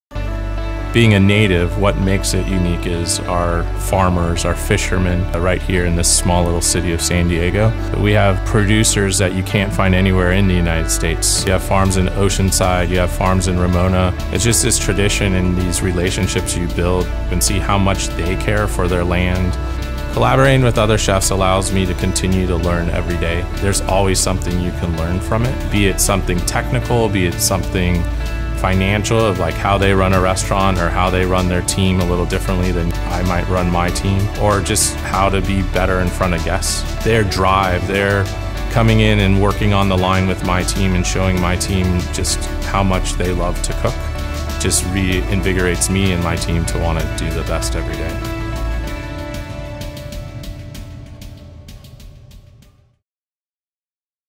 Aron Shwartz - San Diego Bay Wine + Food Festival
Chef Aron Schwartz of the Marriott Marquis San Diego Marina will be welcoming chefs from across the country to explore their roots and inspirations at Dinner with Culinary Icons on Thursday, November 16 and the Roots Dinner on Friday, November 17 as part of the San Diego Bay Wine + Food Festival.

🎥: by Sam Wells (@swellscreative)

Featuring Chef Aron Schwartz of the Marriott Marquis San Diego Marina (@marriottmaruissd)